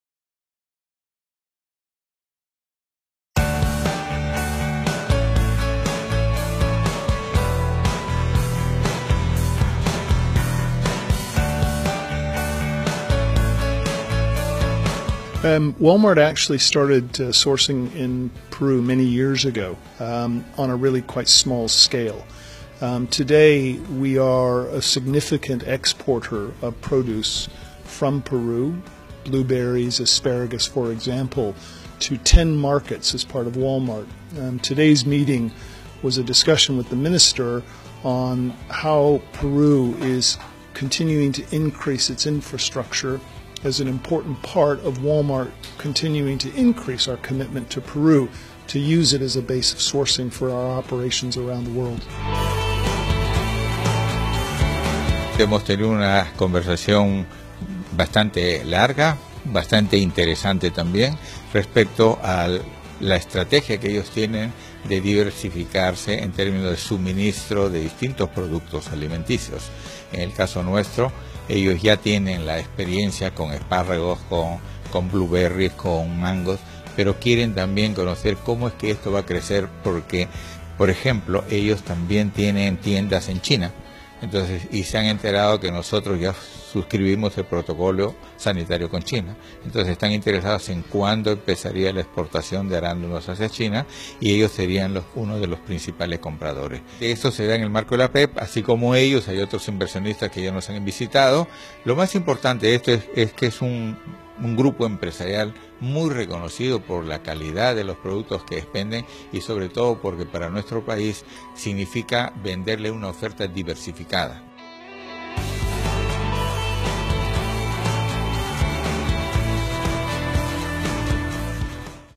Walmart con Ministro Jose Hernandez sobre exportacion de Arandano, y Esparrago
Mercados requieren la produccion de alimentos como Esparragos y Arándanos en el marco que pronto se exportara a China.